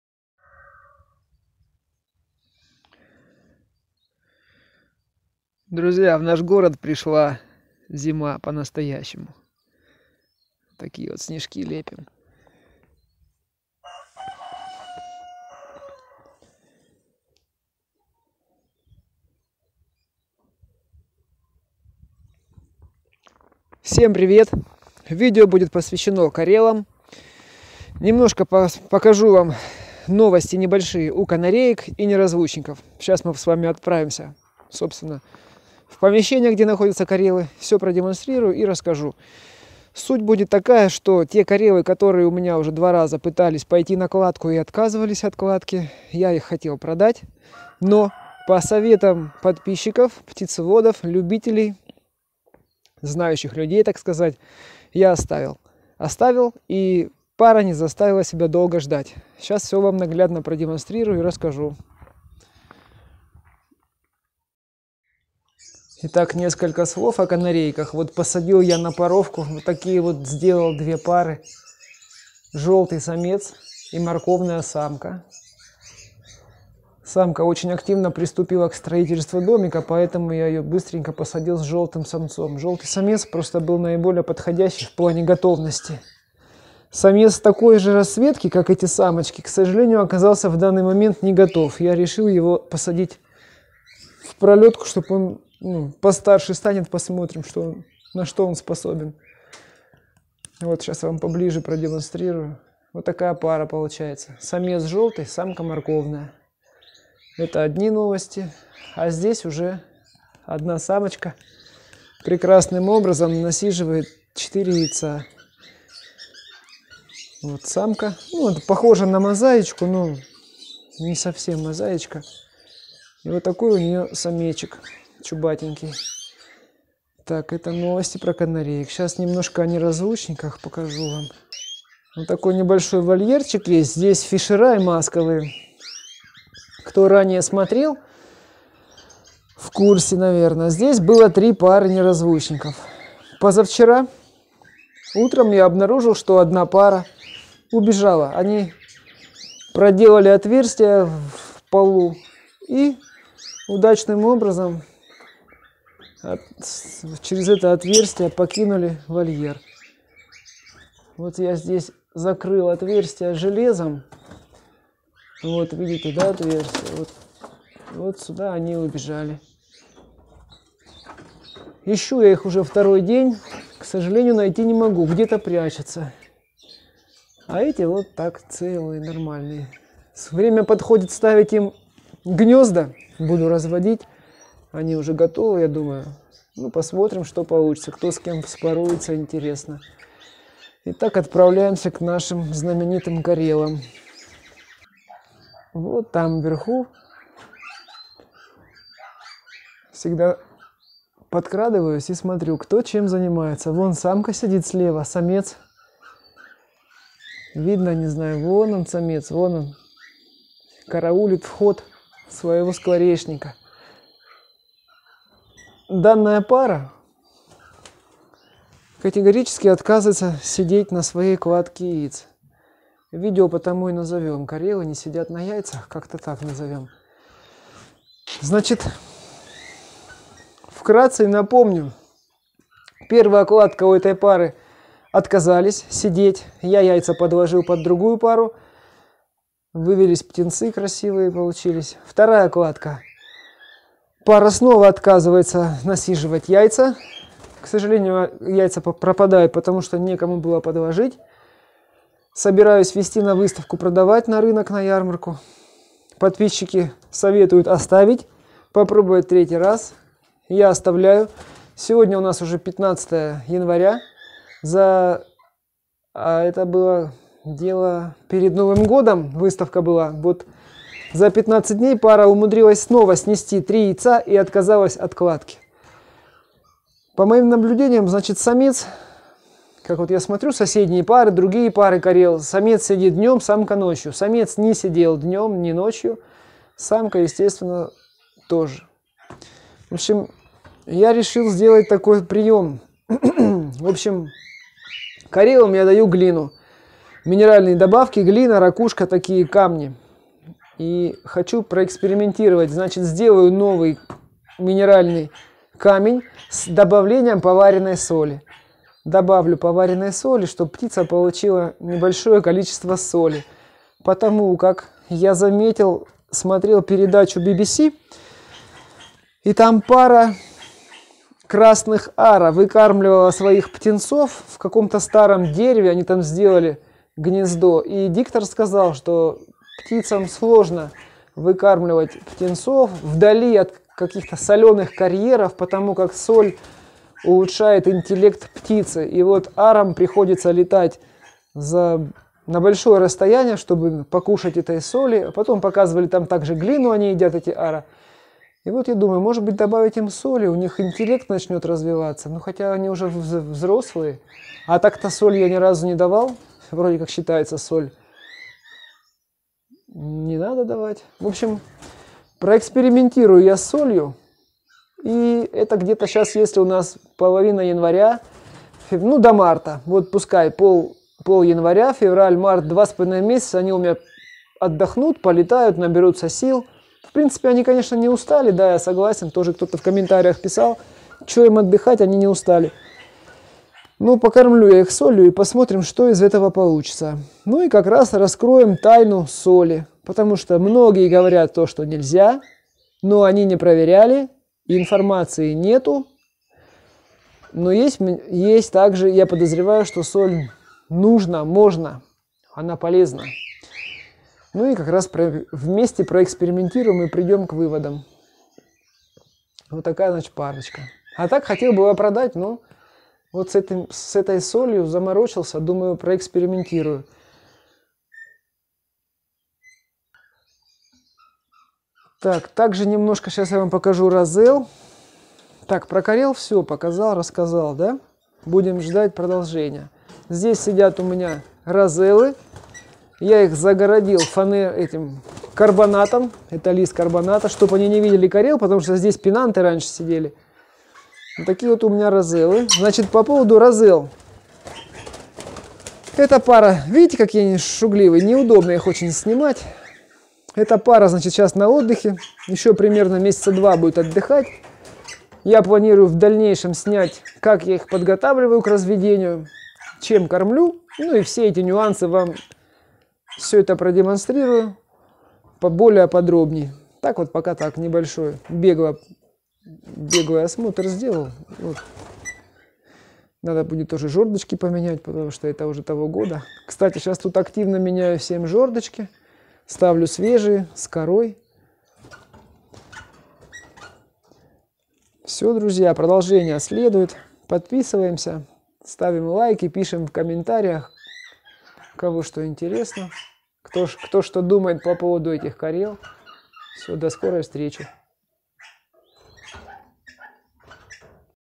Кореллы отказываются от насиживания
видео про пару корелл которая так и не приняла кладку яиц.я дал птицам три попытки но инстинкт насиживания яиц к ним так и не пришел ,причина возможно нехватка соли в рационе.Буду в водить соль затем после отдыха попробую снова !